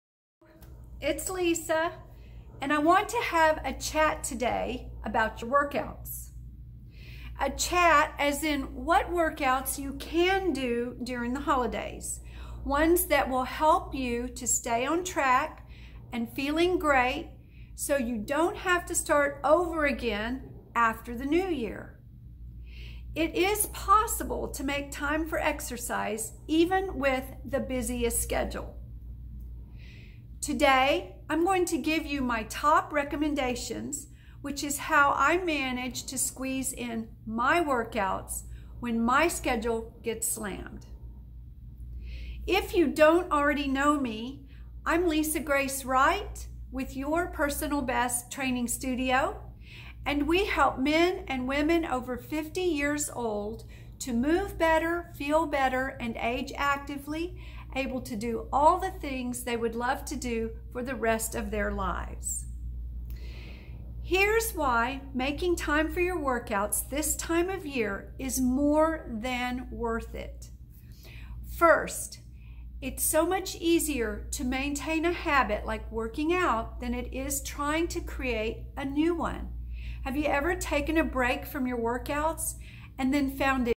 Workouts during the holidays
Join YPB Owner & Functional Aging Specialist, Lisa Grace Wright, as she discusses how you can make time for workouts even during the holidays!

Don't forget to download her Travel Workouts right now as her gift to you for watching!